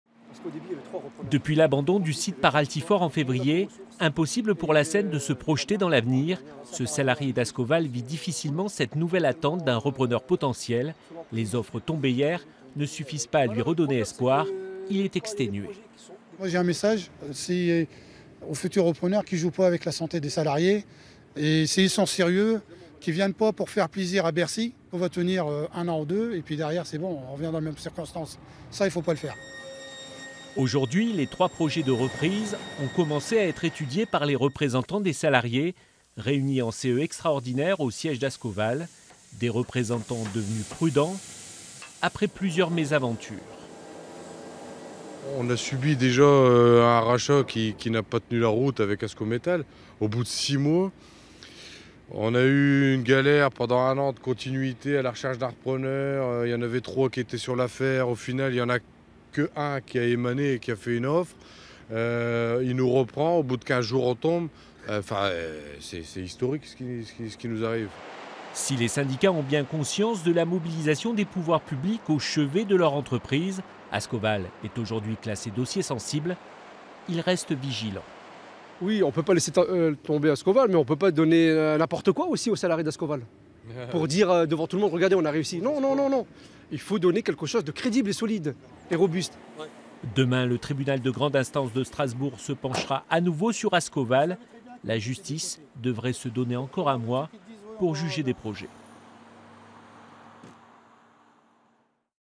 Ascoval : trois offres fermes et une lettre d'intention pour la reprise du site de Saint-Saulve
Plusieurs de ces offres sont jugées "solides".